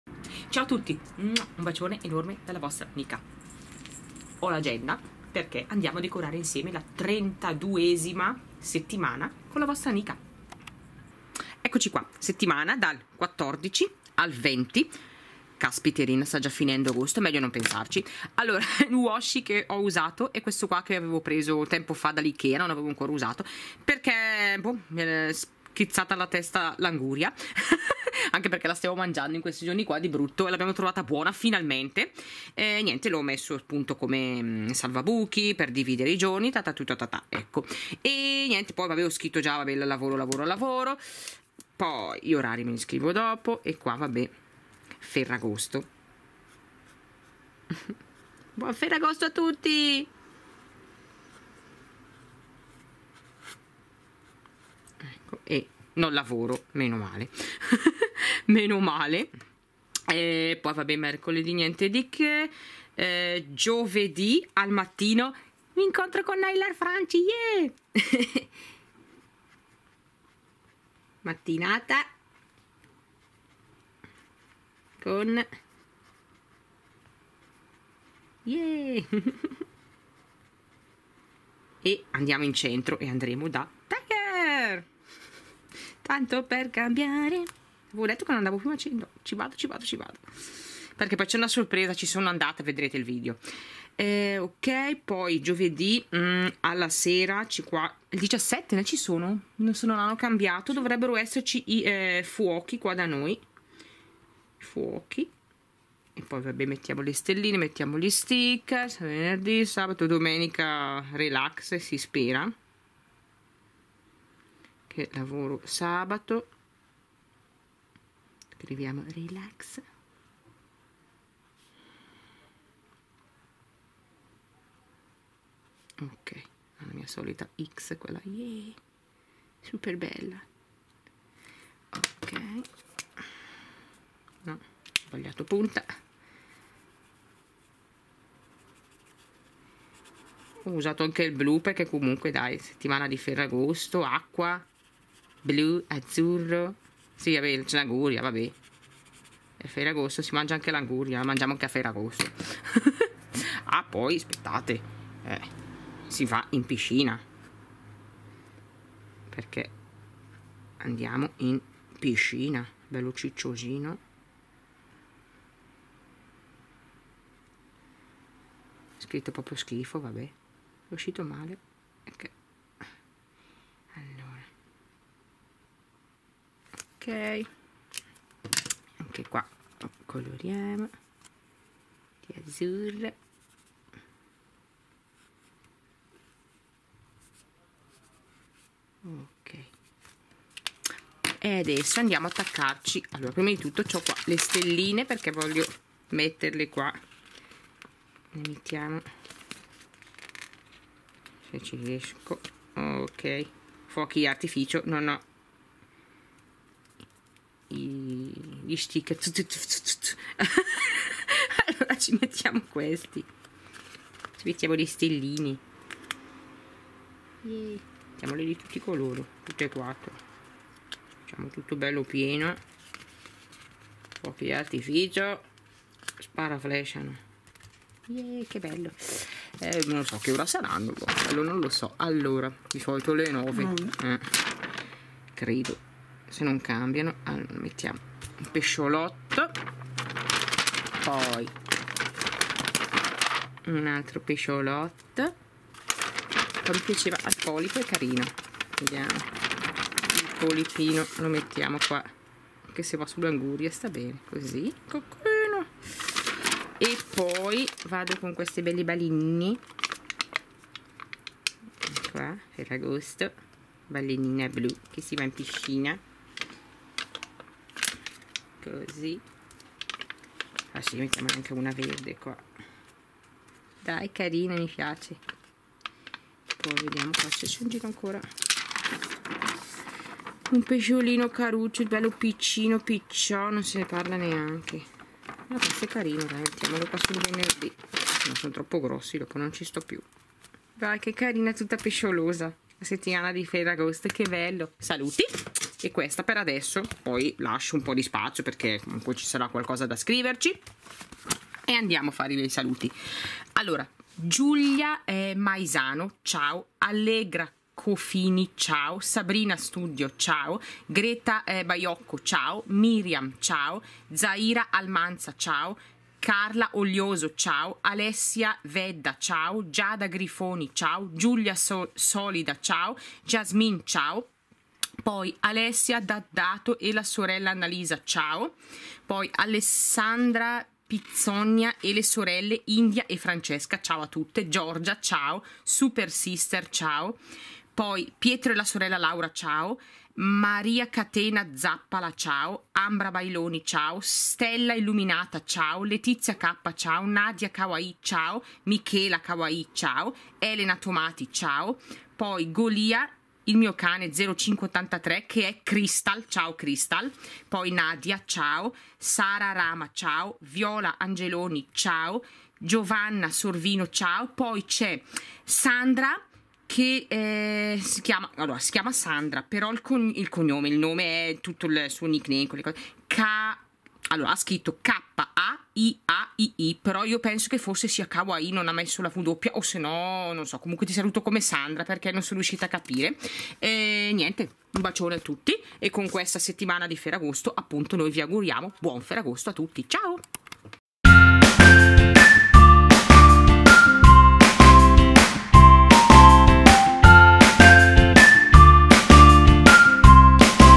Plan With Me #32 - Come decoro la mia agenda Dokibook - By Nika
Ciao a Tutti, oggi siamo alla Settimana 32  di Plan With Me! Settimana che va dal 14 Agosto al 20 Agosto 2017.